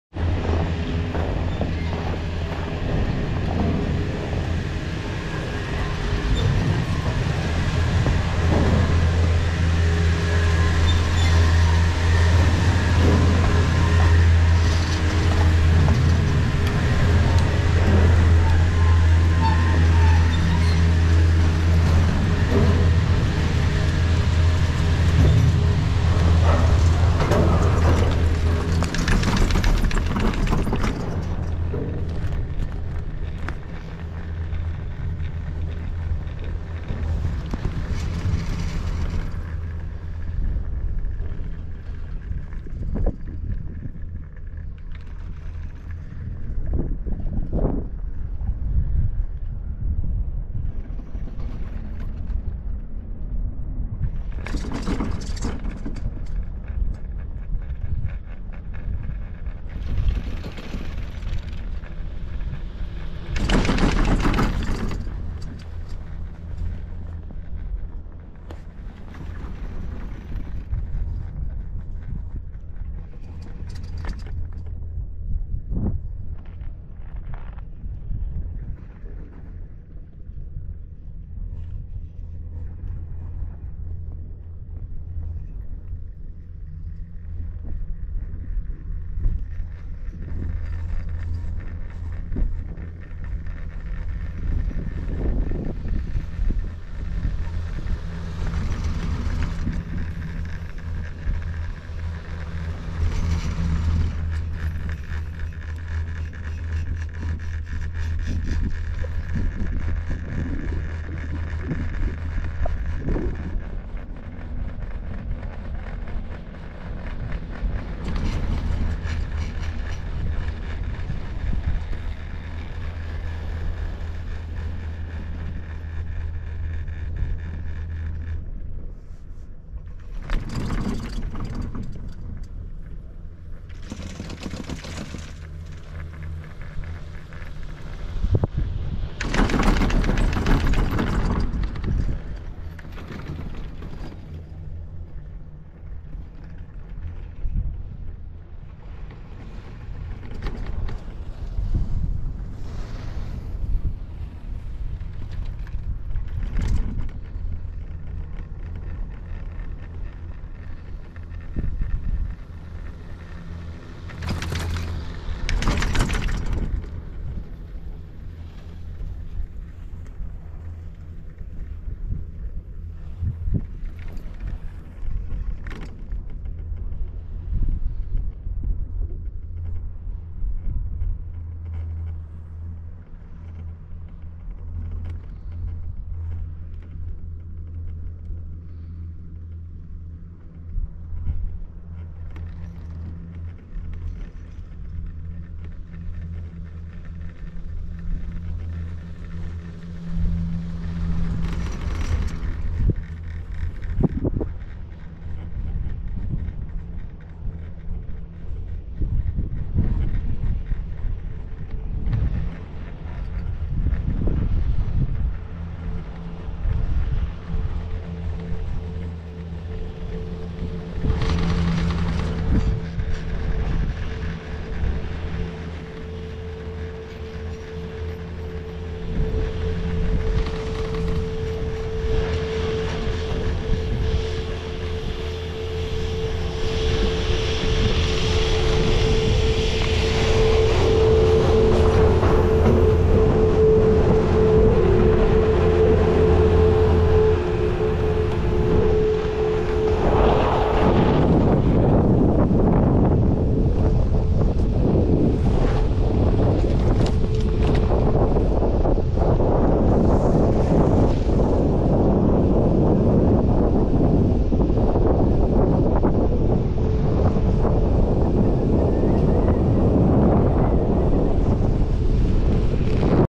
Crystal Mountain | Green Valley
Green Valley was installed in 2000, replacing a Riblet double of the same name (Chair 3), dating back to 1963. This lift is a Doppelmayr high-speed quad, one of the last Spacejets to be installed, being one of the five installed in 2000, and the last six were installed in 2001, before the Spacejet was retired. Green Valley is the shortest detachable lift in Washington, running alongside the longest, Mt. Rainier Gondola. It is also the 88th shortest detachable lift in North America. For a more detailed description of Green Valley, check out the description of this video from earlier this season

Timestamps:
0:00 Bottom Terminal
0:28 Ride Up
4:02 Top Terminal

Recorded on March 22nd, 2023
GoPro Hero 10 Black, 4k, 30fps, Horizon Leveling